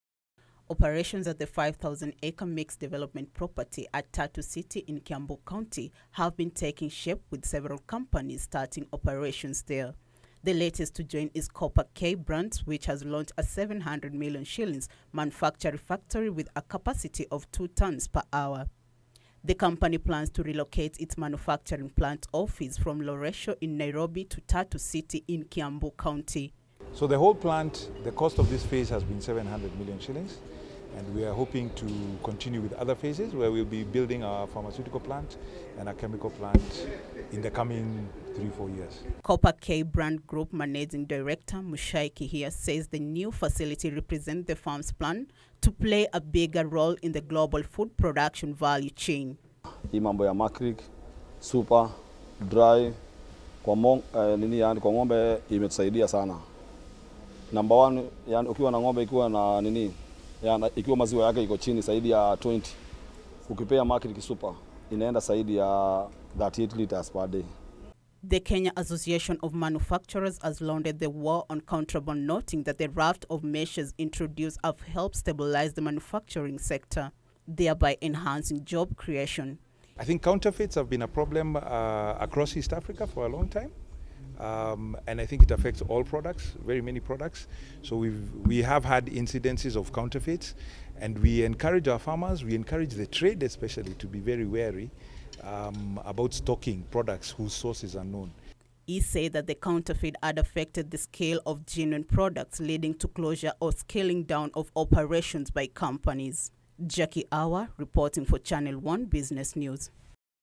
Cooper K-Brands launches a Shs 700 million manufacturing factory at Tatu City
Cooper K-Brands is relocating its manufacturing plant from Loresho, Nairobi County to Ruiru following the launch of a 700 million shillings manufacturing factory at Tatu City, in Kiambu County. Speaking during the launch, manufacturers lauded the government for initiating the war against contrabands that Kenya Association of Manufacturers Vice-Chairman Mucai Kunyiha says had affected the sale of genuine products.